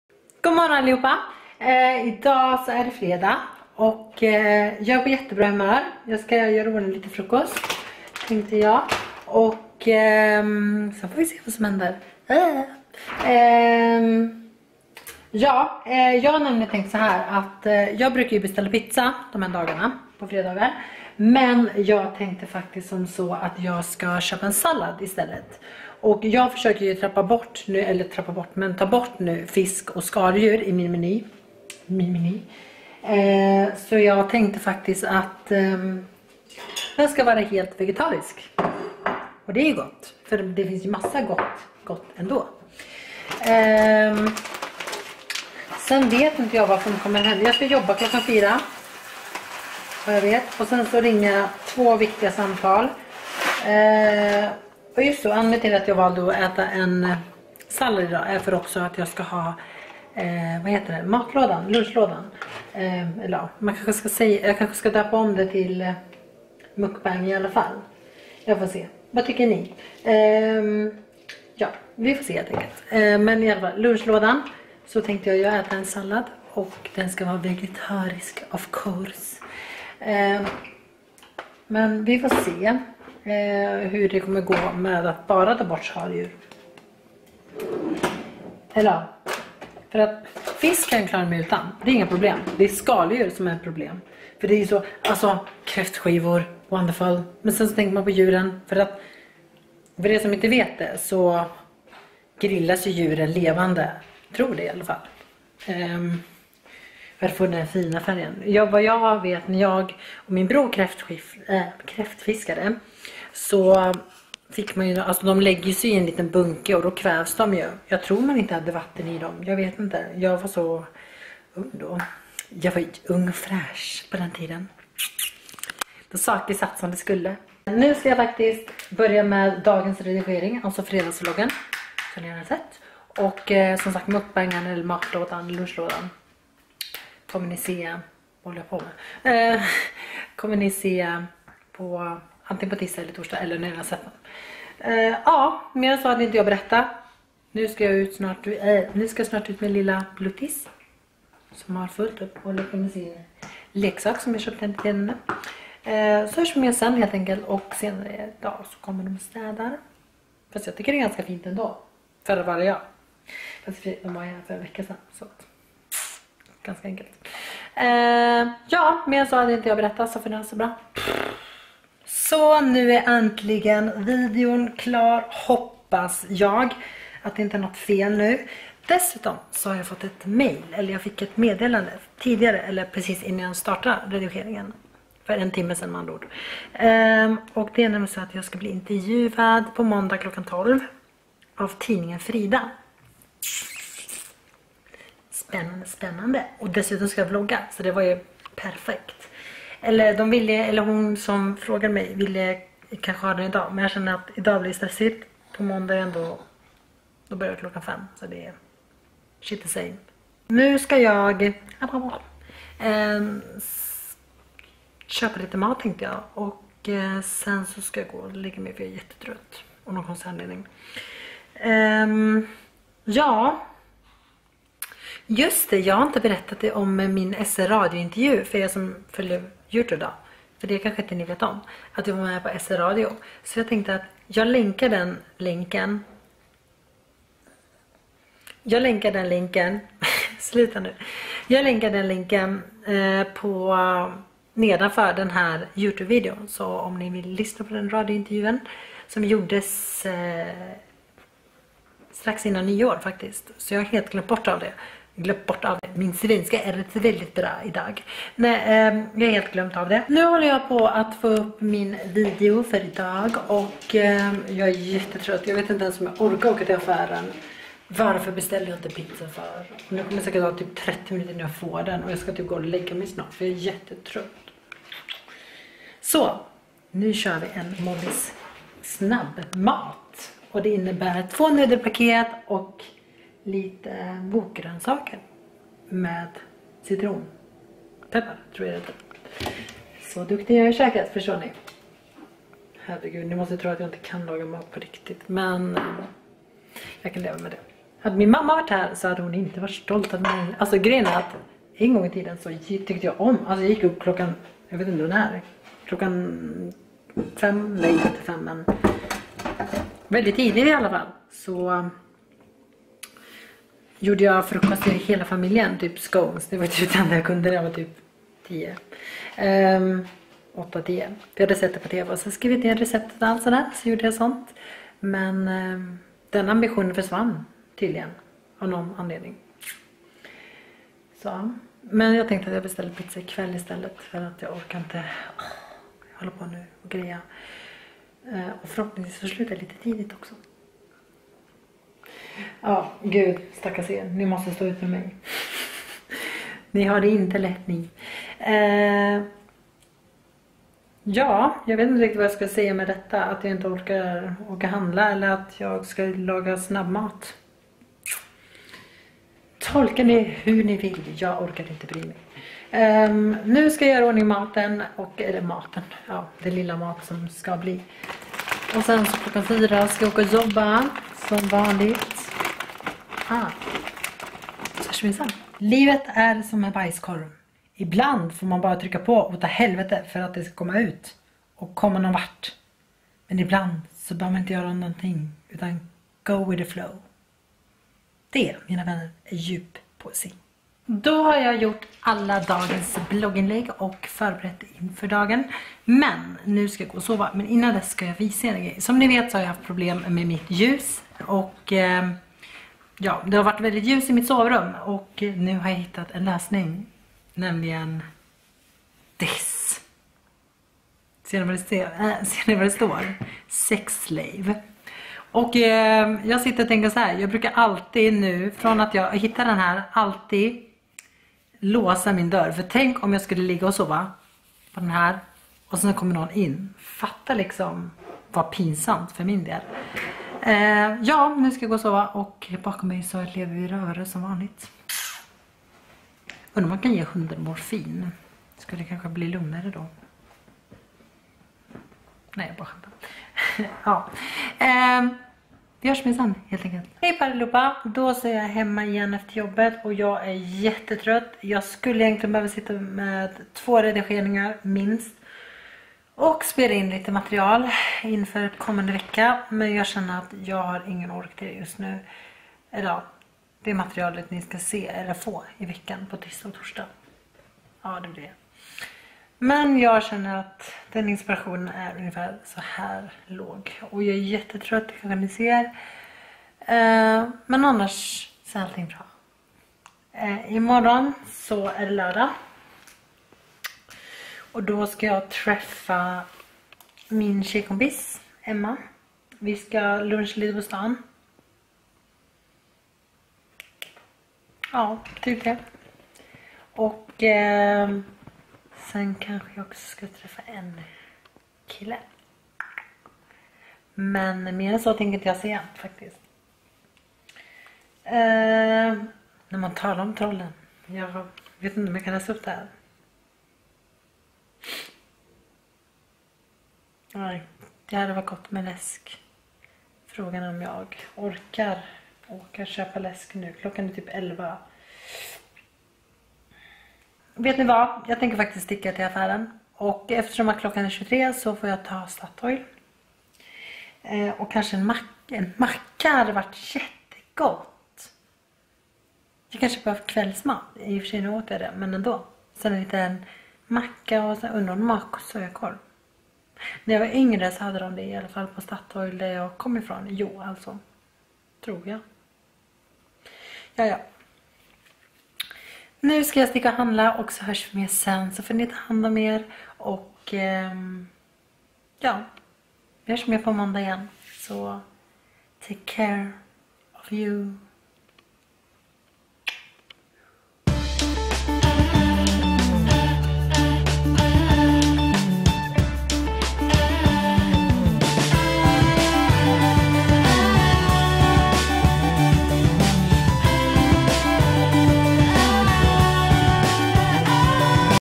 Fredagsvlogg / Intervju på måndag
Hittade inte intervjun från 28 december, men hittade den jag gjorde i mars, samma år, notera att det var innan min näsplastik